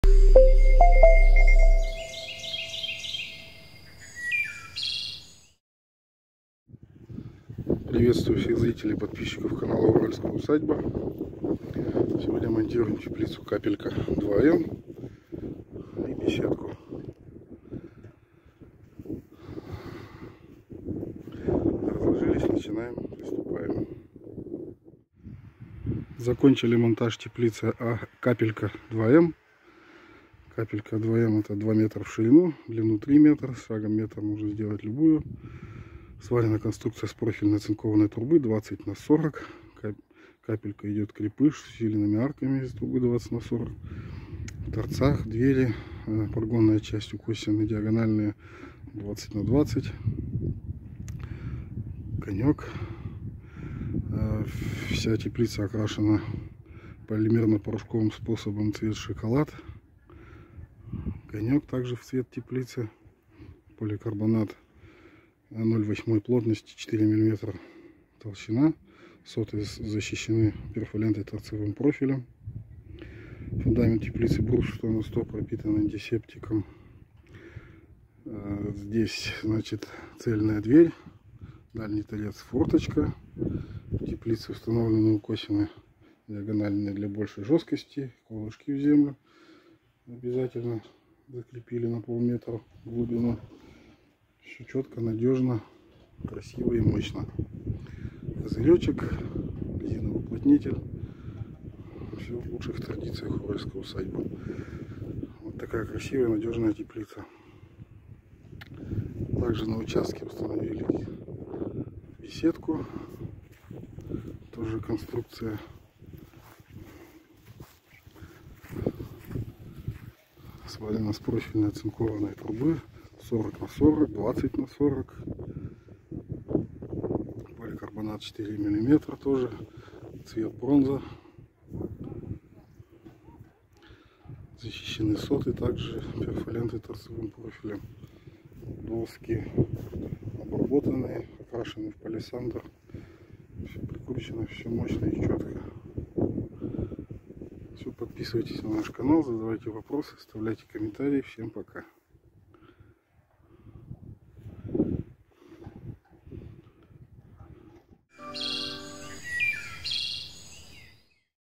Капелька 2М. ширина 2 метра. Беседка садовая.
Как избежать проблем.

Вы часто рассказываете мне истории, что теплицы ломаются от снега. Ну что я могу сказать: «Покупайте качественные теплицы у проверенных производителей». С нашими теплицами такого не случается.

Расскажу на примере теплицы «Капелька 2М крепыш»:

У этой теплицы каплевидная форма, снег на ней не задерживается.

Но даже если каким-то чудом он налипнет, то ничего не случиться,  потому что у этой теплицы особо усиленный каркас, арки и торцы изготовлены с усилителями из оцинкованной профильной трубы 20*40*1,2-1,4 и 20х20х1,2 мм.
Каркас изготовлен из оцинкованной профильной трубы с полимерно порошковой окраской, цвет 8017 шоколад.

Сварка полуавтоматом в среде защитных газов, чистый шов.

Покрыта теплица качественным сотовым поликарбонатом от 4 мм.

Все эти условия позволяют выдерживать снеговые и ветровые нагрузки без ограничений.

Ещё у этой теплицы двойной конек из оцинкованной стали - дождь будет тоже не страшен.

Размеры теплицы «Капелька 2М крепыш»: ширина 2000 мм, длина любая с шагом 1 м. Высота в коньке 2300 мм.

🏡 Екатеринбург ул Климовская 23
🖥 ural-usadba.ru